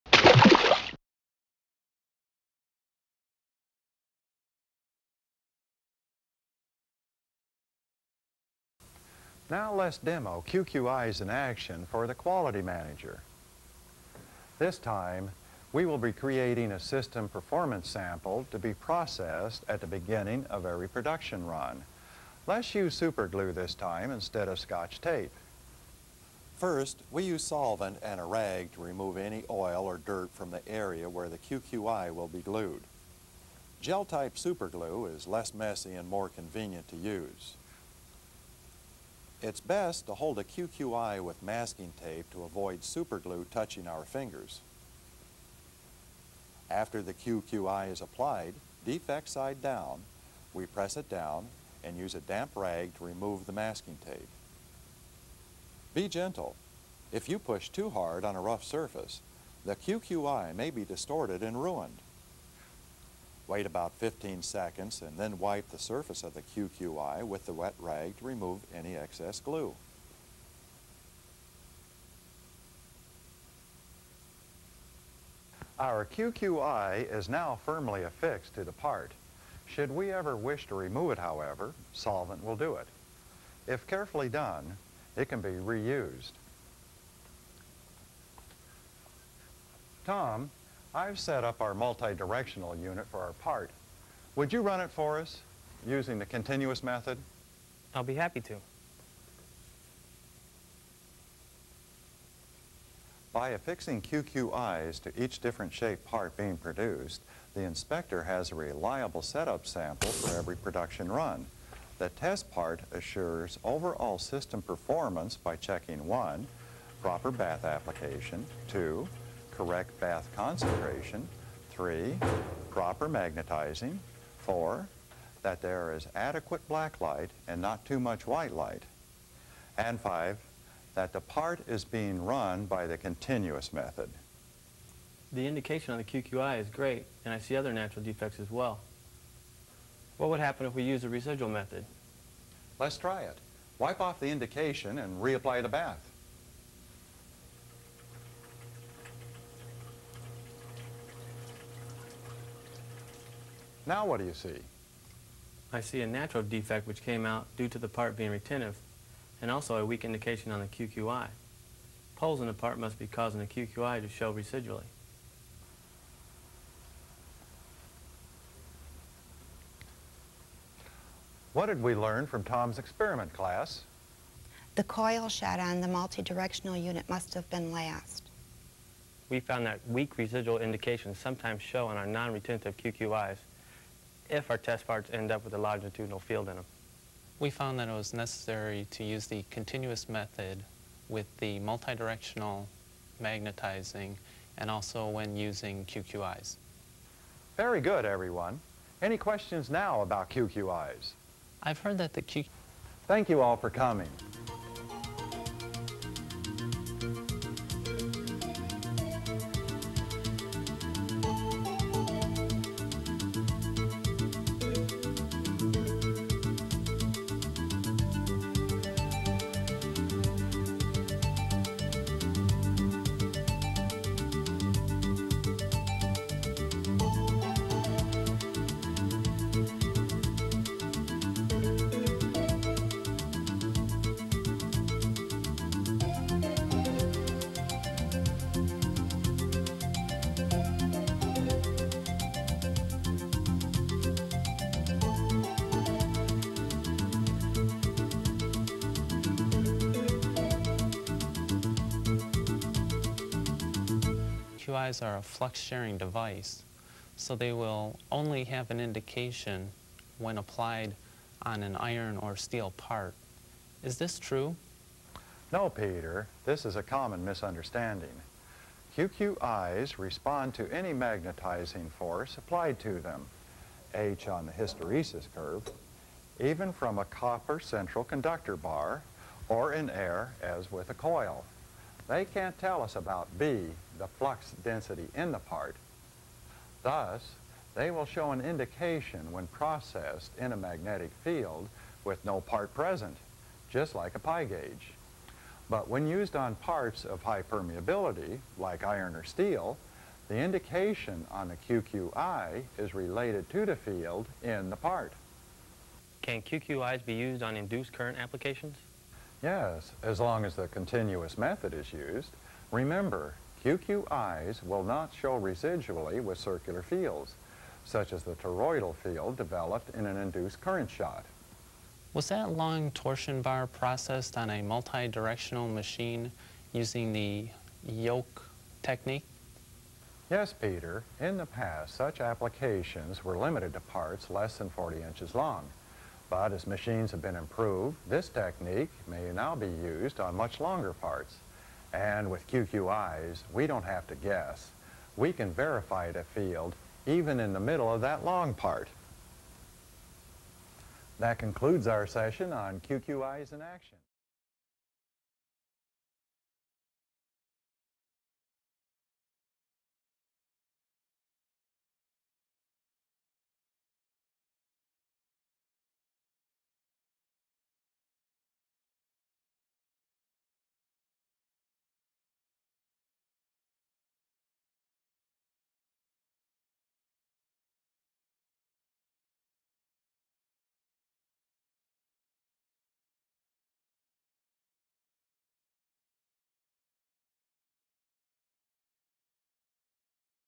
QQI's Part 2.mp4
How to properly use QQI's (artificial flaws) in Magnetic Particle Inspection.